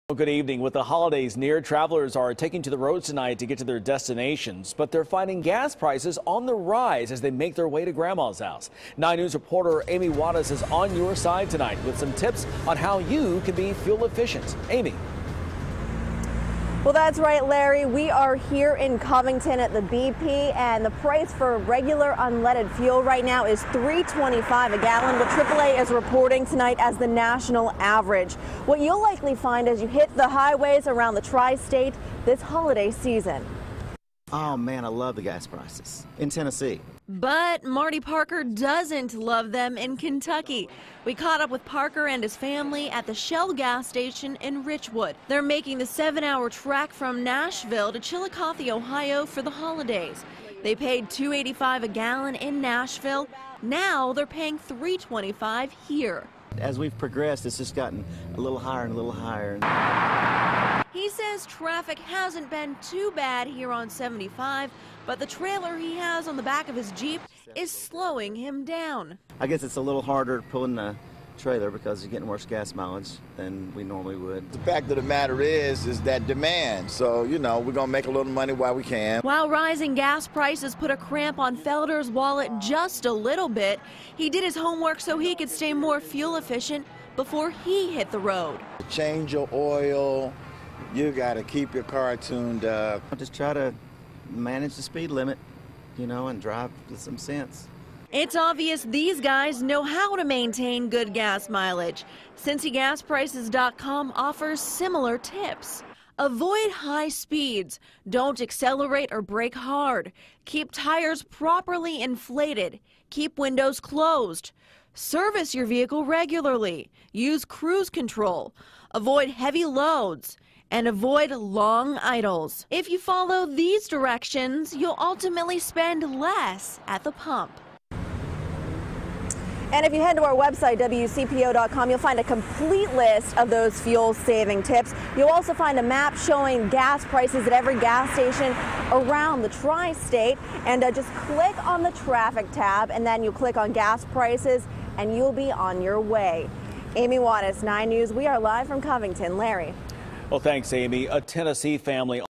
Gas Prices in Tri-State on the Rise
AAA is reporting the national average for regular unleaded fuel to be $3.25 a gallon.